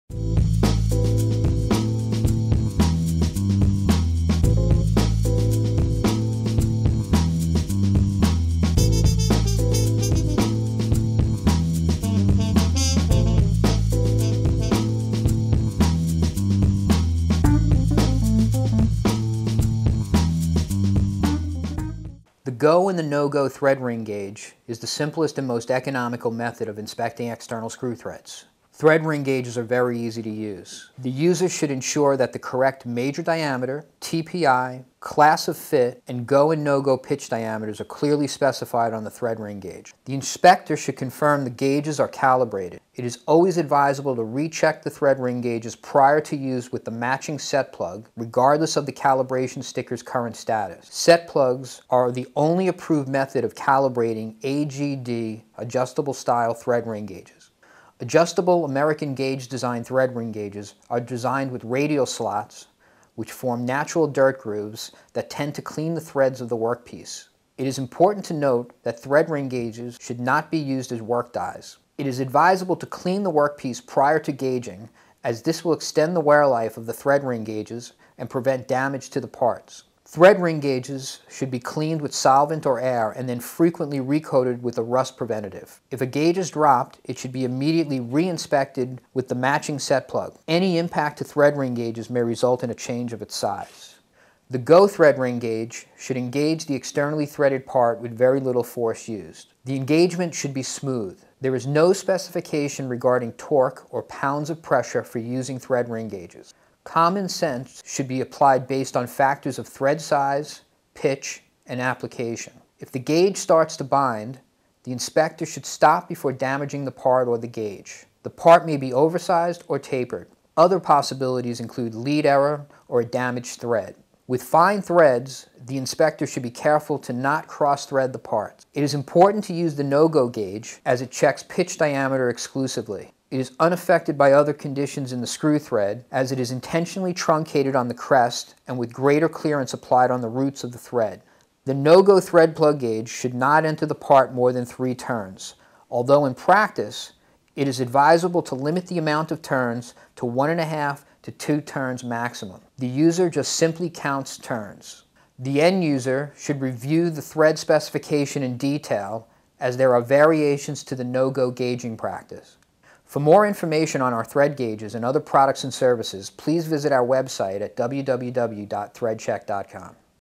How To Use Thread Ring Gages Technical Series 101
The Go and No Go Thread ring gage is the simplest and most economical method of inspecting external screw threads.
Thread ring gages are very easy to use.
The user must first select the correct size thread gage for the corresponding externally threaded product.
Thread ring gage sizes should be clearly labeled on the face of the gage. Green sealing wax is used to seal the adjustment screws for the go member and red wax for the no go member
The user of the thread ring gages should ensure that the correct major diameter, TPI/pitch, Class of fit, and pitch diameters are clearly specified.
It is important to confirm that pitch diameters on the gages match the part specification.
In certain cases the gages may be marked with a P/P or B/P to indicate preplate or before plate sizes.
A part gaged prior to plating will have special pitch diameters. The thread ring gages selected should be engineered and made correctly to the preplate or before plate pitch diameters.
 An “S” after the Unified designation indicates that the thread size is special. A “J” in the Unified designation means that the external thread being inspected has a modified root radius.

The user should also confirm that the gages conform to the proper national or international standard.
In the USA adjustable style thread ring gages are supplied as the standard. In other countries the solid style thread ring gage is considered. The advantages and disadvantages of both is explained in our technical section at threadcheck.com
Finally the inspector should confirm the gages are calibrated. It is always advisable to recheck the thread ring gages with a matching setplug gage prior to use regardless of the calibration sticker’s current status.
Many companies arbitrarily select annual frequencies of calibration rather than basing calibration frequency on usage. This can be dangerous as thread ring gauges can be worn out of tolerance or accidentally dropped and knocked out of size.

The best thread gaging practice utilizes several sets of thread ring gages. One or two gages can be used for in process inspection while another gage can be used for final inspection and a master thread ring gage retained for dispute in the event that gages fail to correlate on acceptance or rejection. Its ideal if all the gages can be set to the same setplug.
A clean thread prior to gaging will provide the inspector with more sensitivity of thread fit. Thread ring gages should be cleaned with solvent or air and then frequently recoated with a rust preventative/gage preservative. In clean room environments Isopropyl alcohol may be substituted to help reduce friction on the gages. Ring gages should be turned onto the parts with care. They should never be hand spun onto the part.

Thread ring gauges should be protected in plastic cases or coated in wax when not in use to prevent nicks to the threads. All gauging whenever possible should be done over an inspection table in the event that the gage is accidentally dropped. If a gage is dropped it should be immediately re-inspected with a setplug gage.

The go thread ring gage should engage the external threaded part with very little force used. The engagement should be smooth. There is no specification regarding torque or pounds of pressure for using thread gages. Common sense should be applied based on factors of thread size, pitch, and application. If the gage starts to bind, the inspector should stop before damaging the part or the gage. The part may be oversized or tapered. Other possibilities include lead error or a damaged thread. With fine threads the inspector should be careful to not cross thread the parts.
It is important to use the no go gage as it checks pitch diameter exclusively. It is unaffected by other conditions in the screw thread as it is intentionally truncated on the crests and with greater clearance applied on the roots of the thread. The no go thread ring should not enter the part more than 3 turns although in practice it is advisable to limit the amount of turns to 1 ½ - 2 turns max. The user just simply counts turns.

The end user should review the applicable thread specification in detail as there are variations to the above practice. /no-go-gaging-per-ansi-asme-b1.2-1983-an-american-national-standard/technicalinfo/.